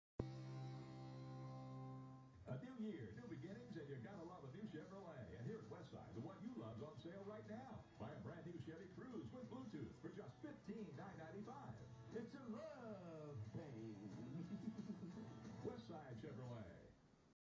Crazy Ass Gary Busey in local Houston commercial
Literally just saw this and couldnlt believe my eyes. 'Ol Busey must be pretty hard up for cash. "IT'S A LOOOOOOOOOOVE THINGGGGGGGAH!"